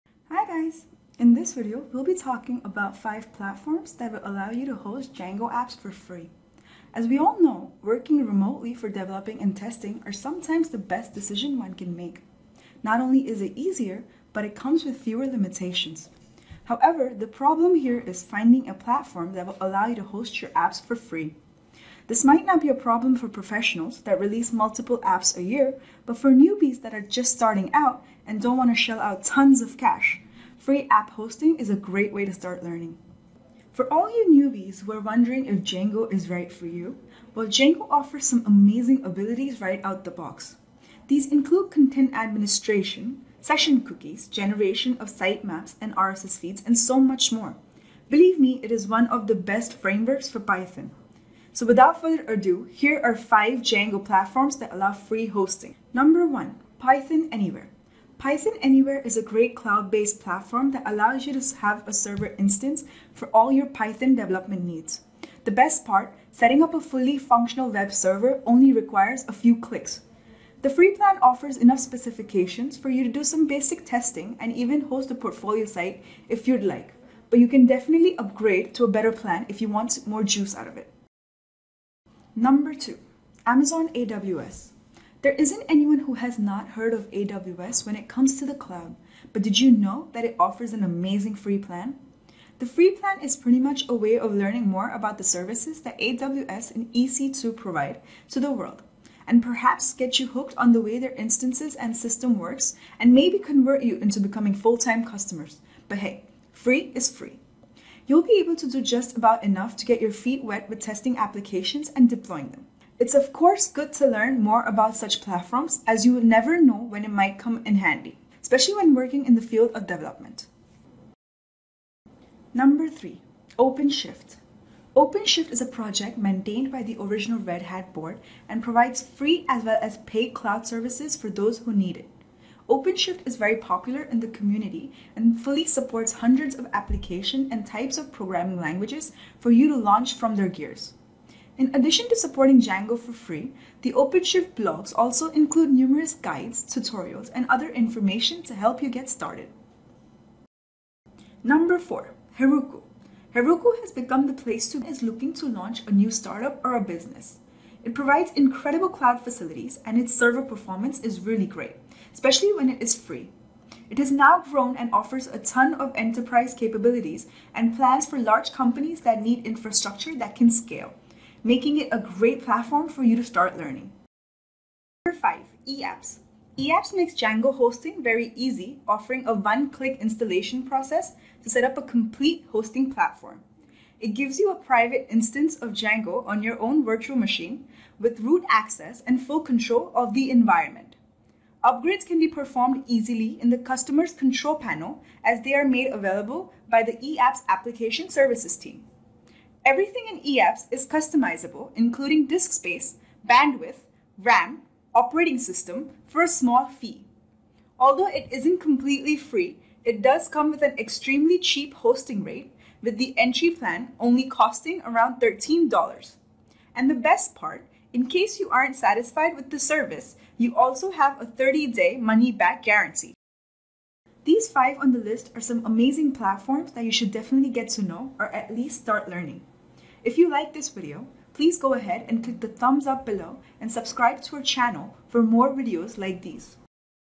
5 Platforms That Provide Free Django App Hosting
Trying to find free web hosting is hardly a problem, as there are plenty of options available for those who want to try remote environment for the first time. It becomes difficult when we need a server that can enable us to run applications like Django, and host apps created through the framework.

In this Blog, You Can Learn about the 5 Platforms That Provide Free Django App Hosting.

Get Any Course for $9 at Eduonix Learning Solutions